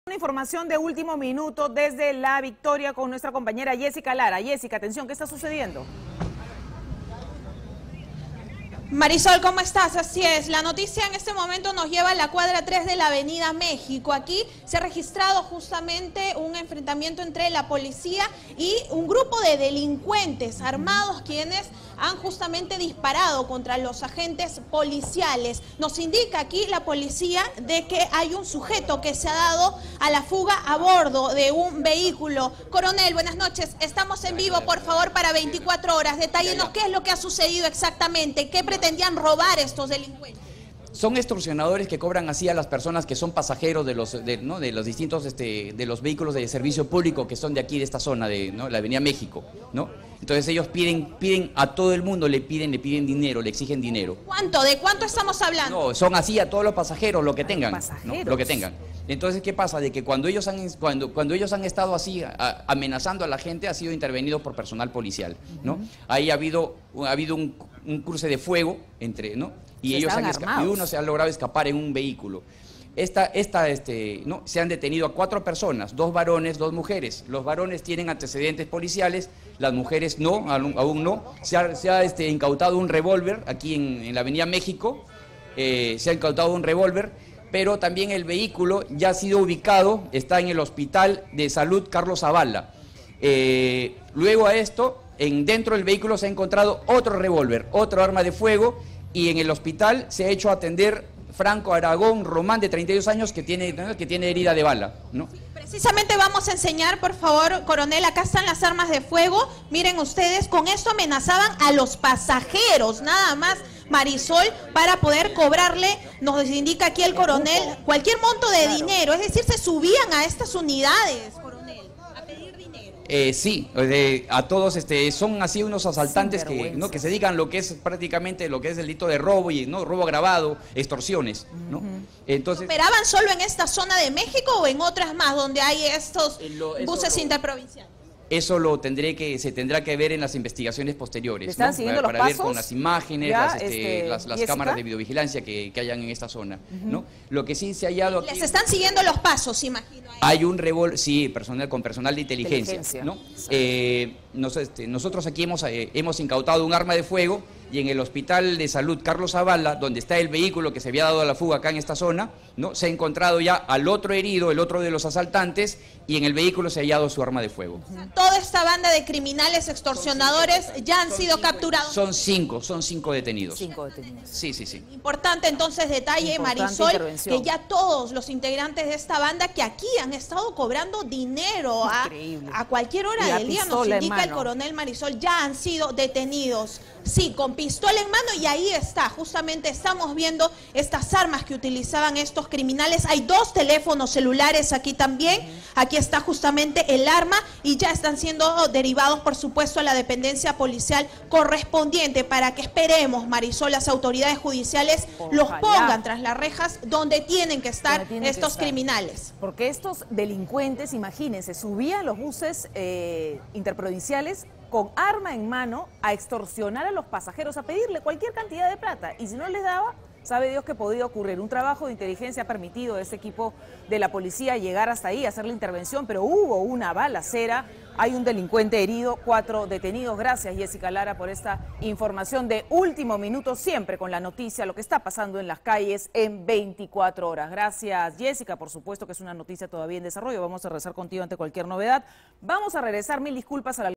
La Victoria: capturan a delincuentes armados tras balacera
Tras una intensa balacera, agentes de la policía capturaron la noche de este miércoles a cuatro delincuentes en el preciso momento en que solicitaban, bajo amenaza, dinero a los pasajeros de los buses de transporte público. Todo ocurrió en la tercera cuadra de la avenida México.

Según las autoridades, estos sujetos eran reincidentes en la zona, por lo que al notar la presencia policial intentaron darse a la fuga. Los agentes fueron detrás de estos criminales, quienes respondieron a balazos desatándose un intenso fuego cruzado por varias cuadras.

Al final, la policía logró capturar a cuatro personas: dos mujeres y dos hombres. Estos últimos registran diversos antecedentes por robo agravado y tenencia ilegal de armas, por lo que fueron trasladados a la comisaría del sector para iniciar las diligencias correspondientes.

UN DELINCUENTE HERIDO

Debido al intercambio de disparos, uno de los delincuentes resultó con una herida de bala. Se trata de Franco Aragón Román, de 32 años, quien fue trasladado de urgencia al centro de salud más cercano para recibir atención médica. Sus cómplices permanecen detenidos en la comisaría.